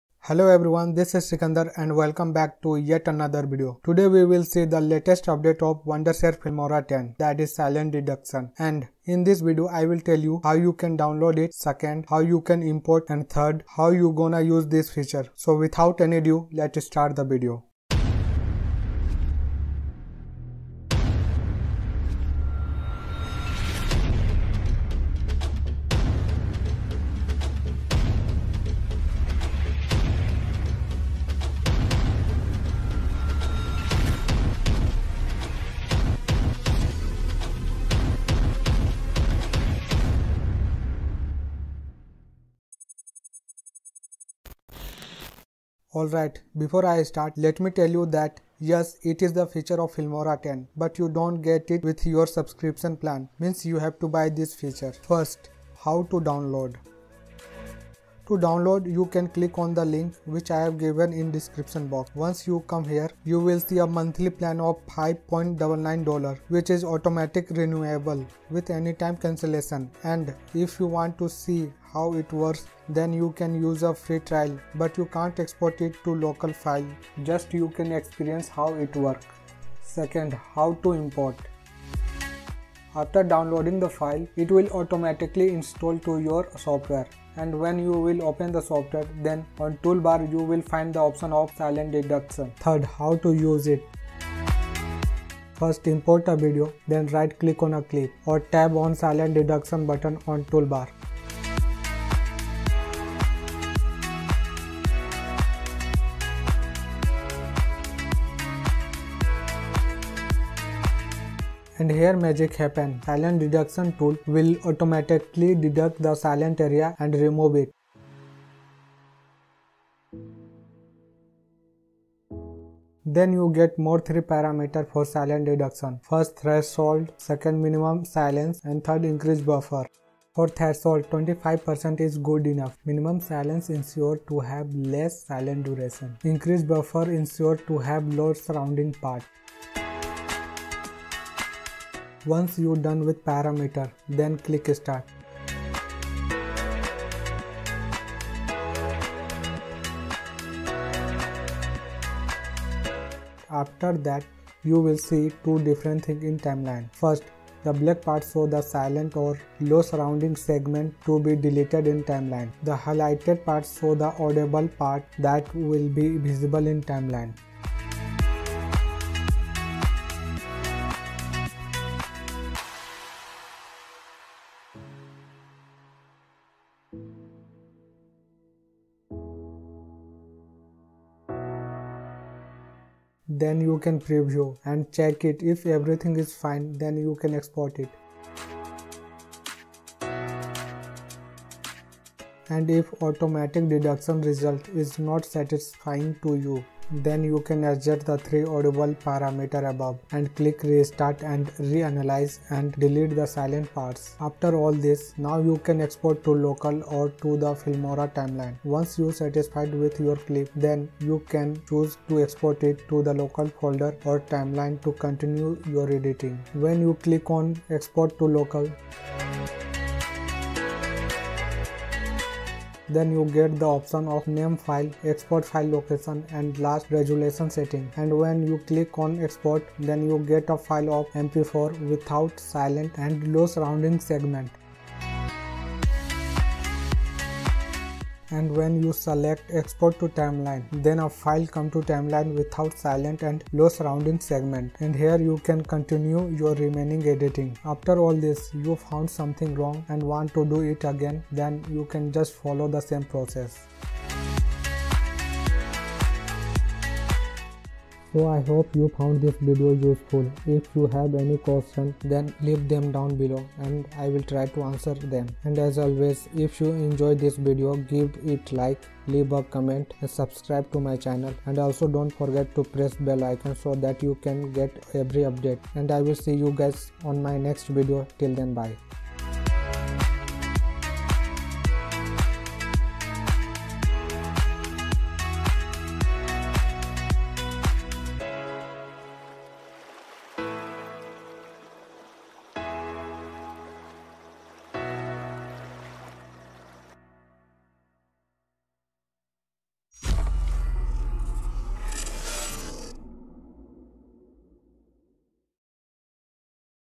ALL ABOUT NEW ADD-ON:SILENCE DETECTION | WONDERSHARE FILMORA X | SKF!
In this video, I will show you new Add-on Feature(Silence Detection) of Filmora X.

Wondershare Filmora X Just released with lots of new features like Motion Tracking, Keyframe Animation, Color Match, Audio Ducking.

If you create something than tag on Instagram @skf.films